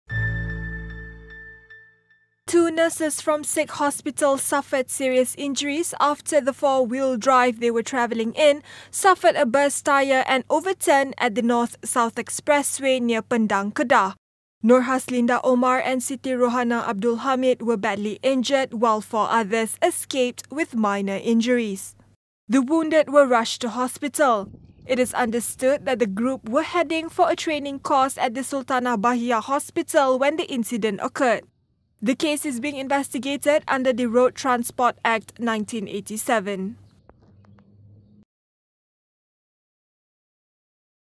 Two nurses suffer critical injuries after 4WD overturns
Two nurses from Sik Hospital suffered serious injuries after the four-wheel-drive they were travelling in suffered a burst tyre and overturned at the North-South Expressway near Pendang, Kedah Sunday morning.